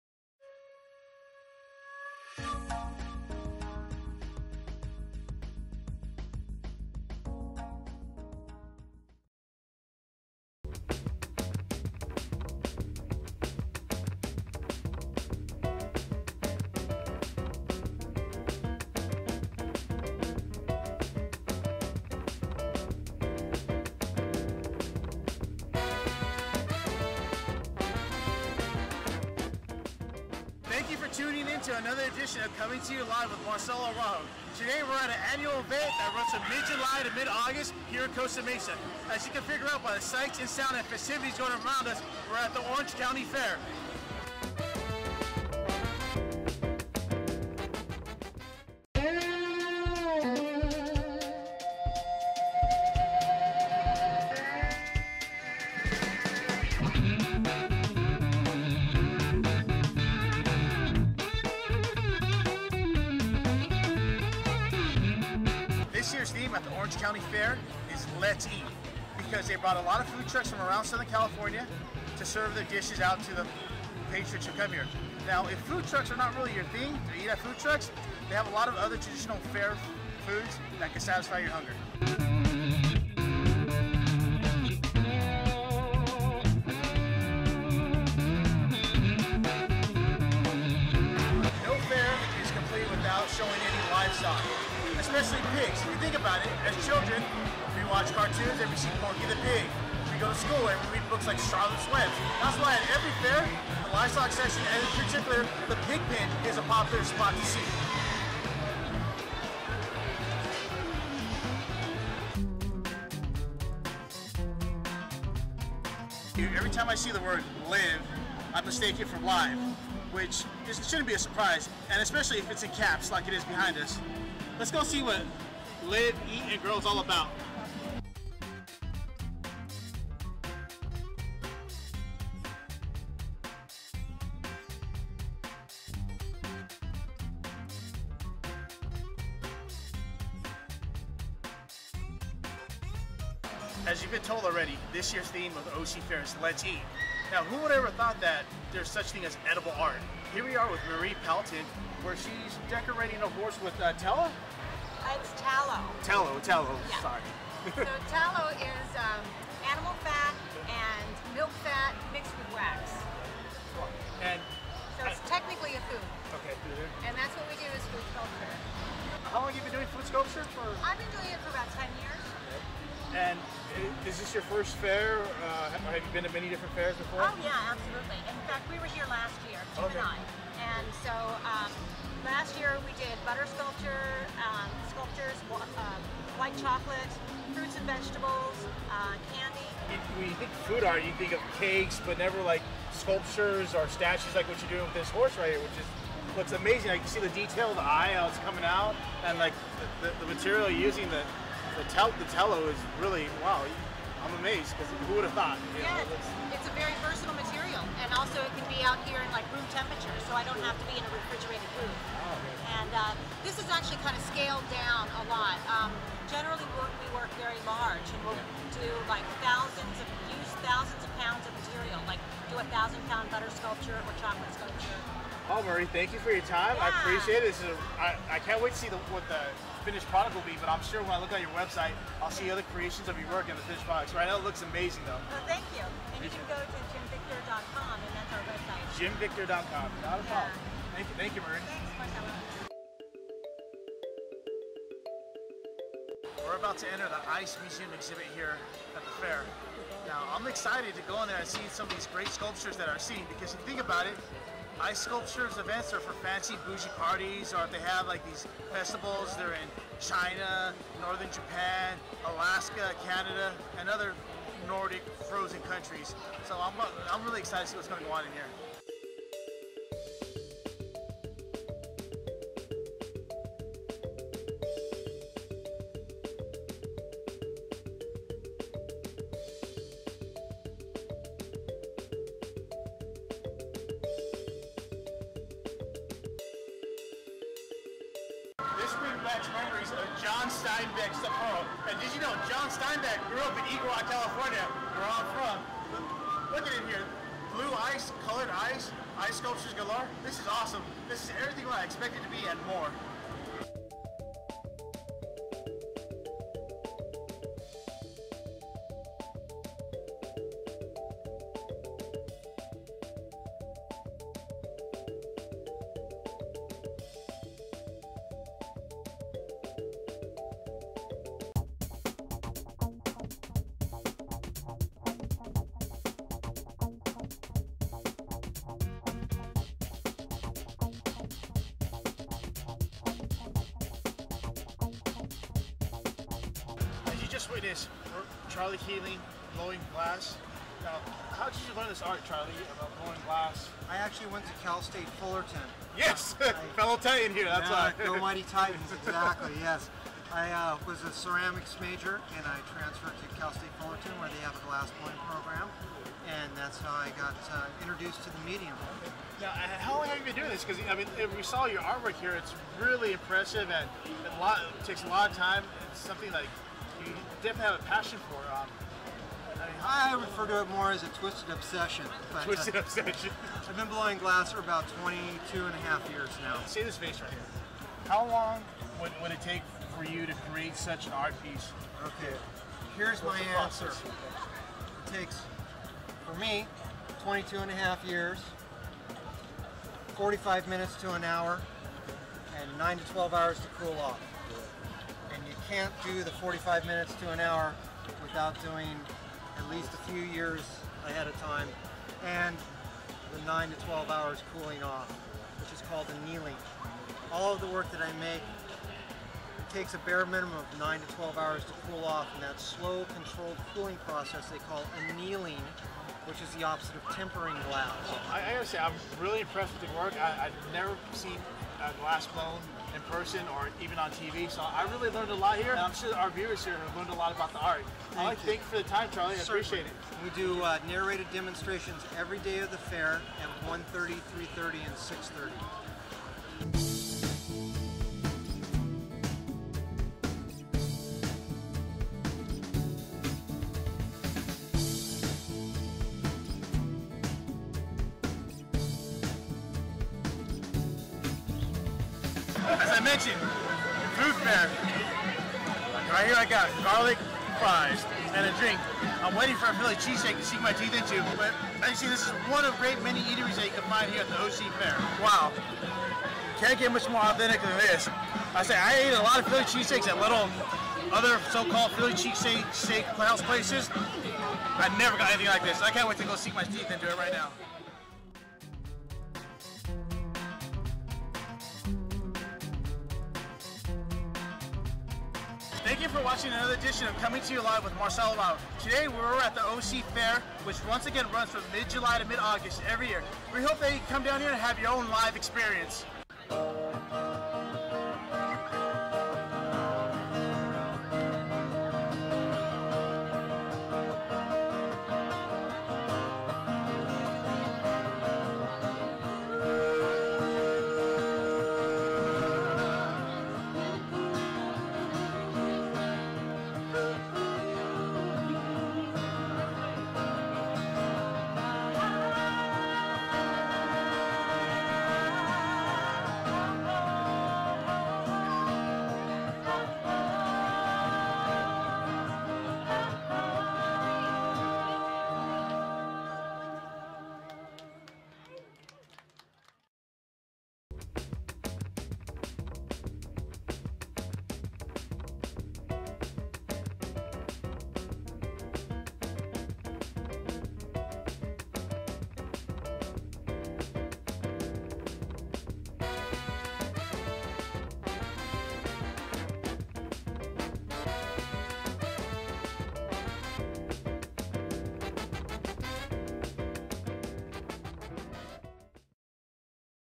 Coming to You LIVE (OCFair)
Follow Marcelo Araujo as he takes you to the Summer Craze known as the OC Fair.  Learn about this years delicious theme and see the wonders that shouldn't be missed.

Host: Marcelo Araujo
Shot/Edited/Music: Manny Montanez

For more info on O.C. Fair, log onto

Featured on today's Episode

Jim Victor Food Sculpture

CJK Blown Glass